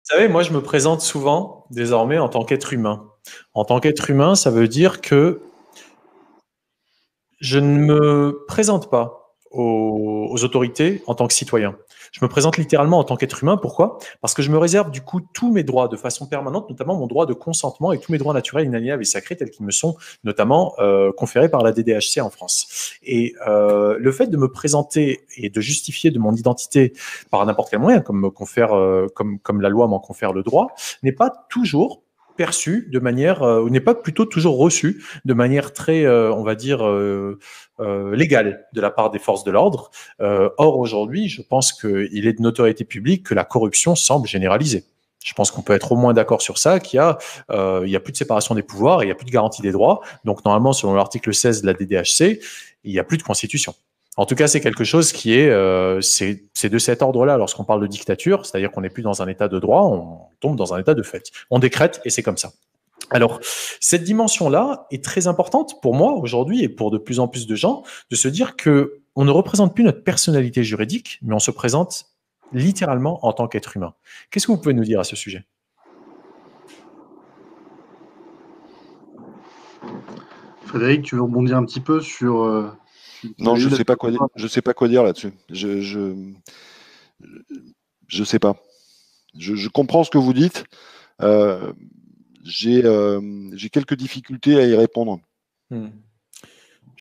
I- On se présente en tant qu'être humain avec Clarisse Sand, Frédéric Niel et David Guyon
On ne se présente plus en tant que personne juridique mais en tant qu'être humain, qu'est-ce que vous pouvez dire à ce sujet ?

Extrait de la vidéo originelle - L'Atelier du Dimanche 6/12 spécial Droits avec Clarisse Sand, Frédéric Niel et David Guyon

No copyright = diffusion libre

Force, courage et santé à tous les humains !